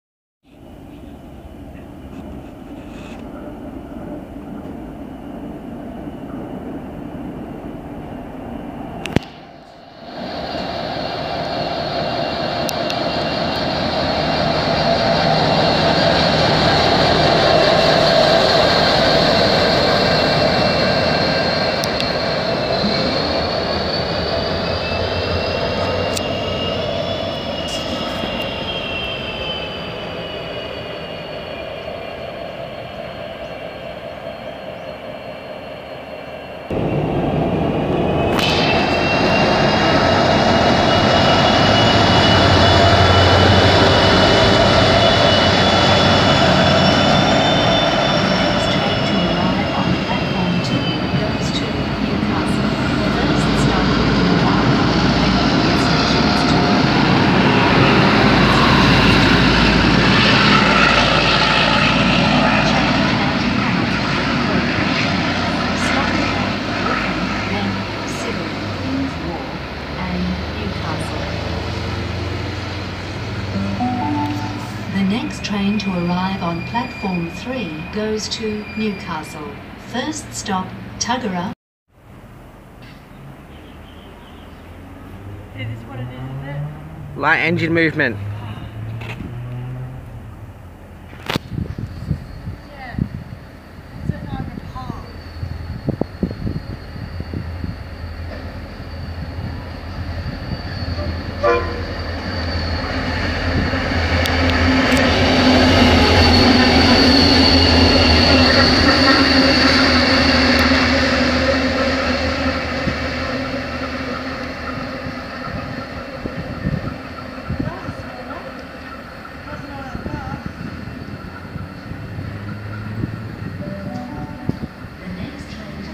SSR light engine movement
Today is my last video for 2 months but the 2 SSR light engine movements were to good to pass up and share with you all

Trains were

D178 SSR102 (Bendigo) heading north
D478  C506-507-BRM001 heading south to join SSR 1845